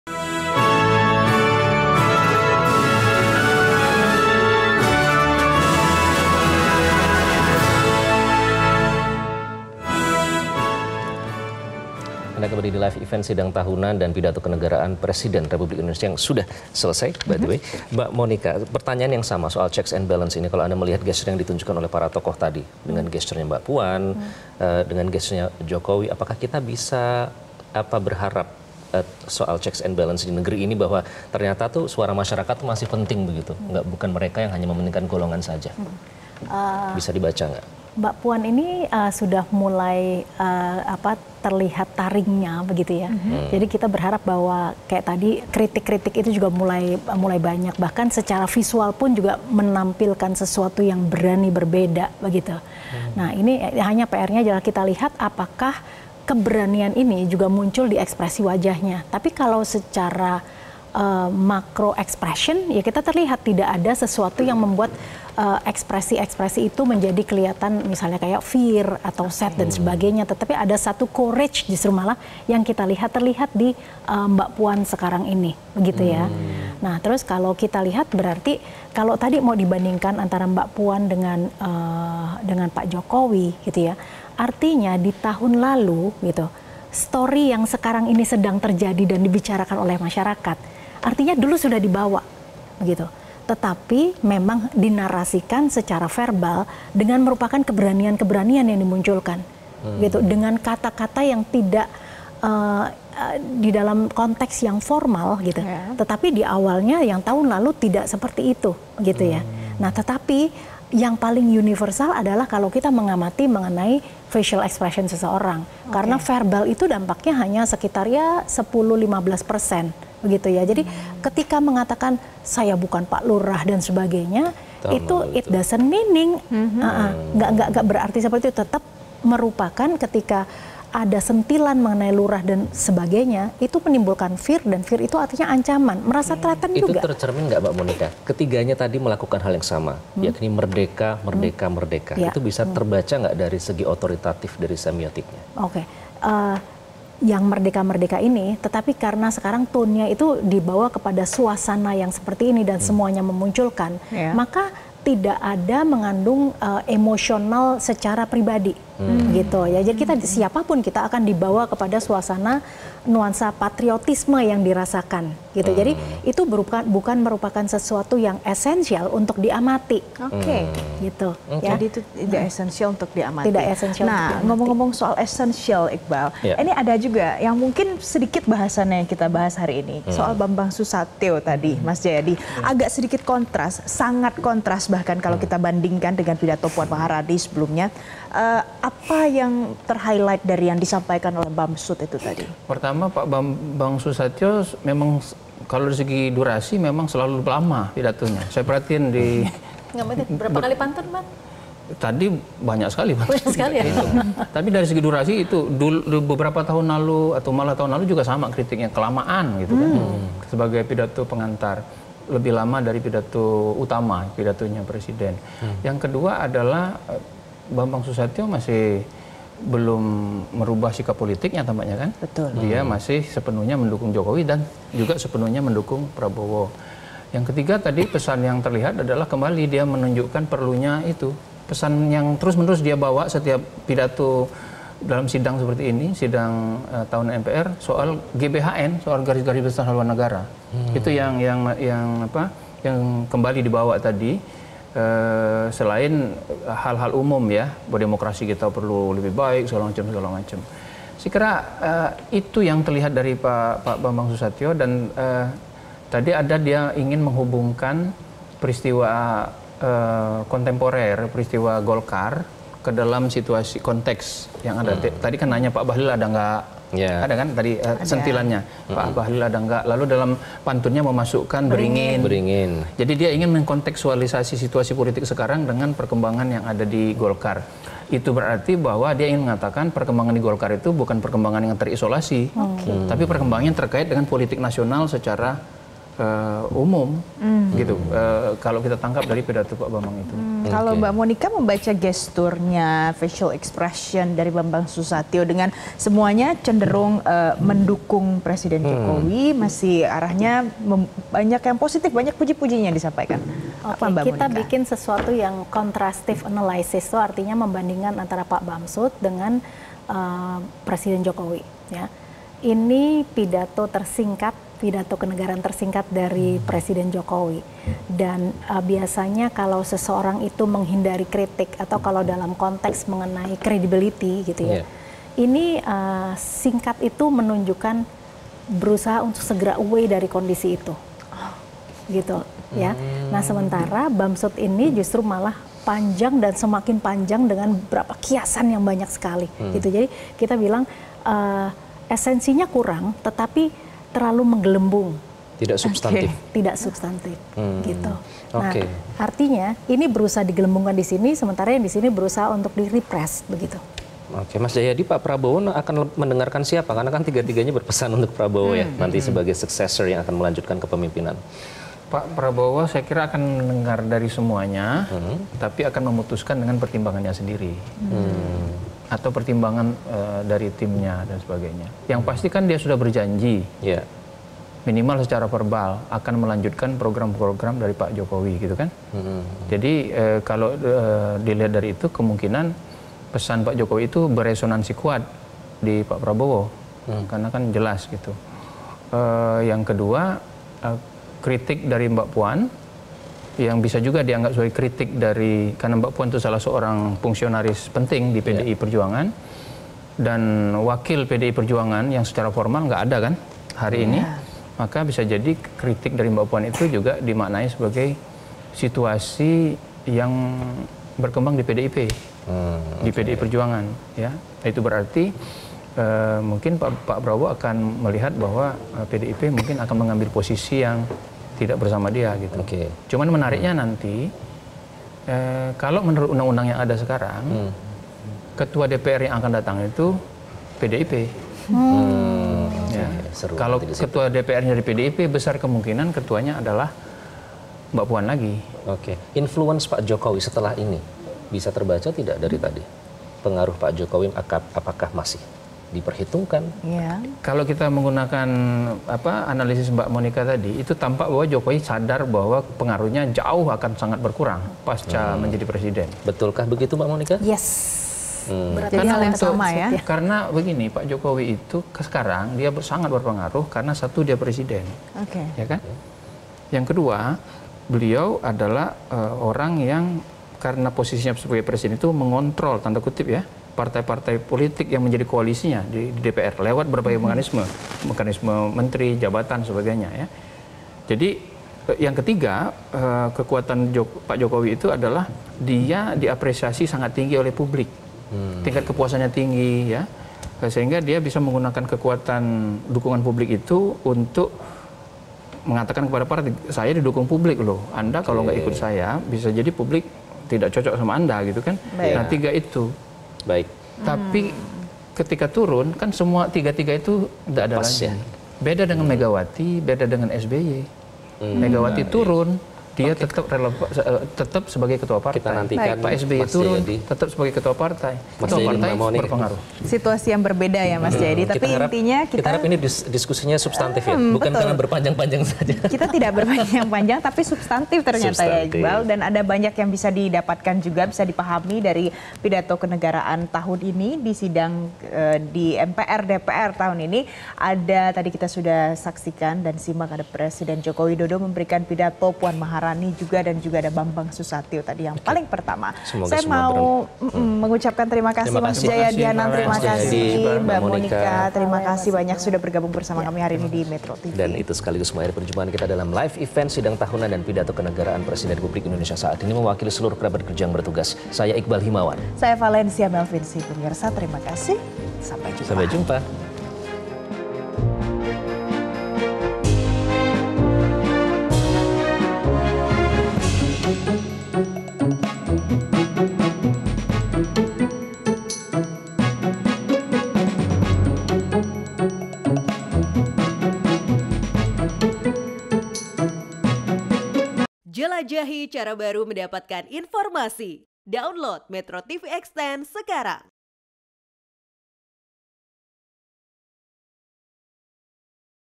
LIVE EVENT - Pakar: Kritik Puan saat Pidato Penuh Keberanian
MetroTV,
Pemerintah menggelar sidang tahunan MPR RI, DPR-DPD RI. Presiden Jokowi dan Menteri Keuangan juga akan membacakan pidato kenegaraan serta pidato pengantar Nota Keuangan APBN 2025.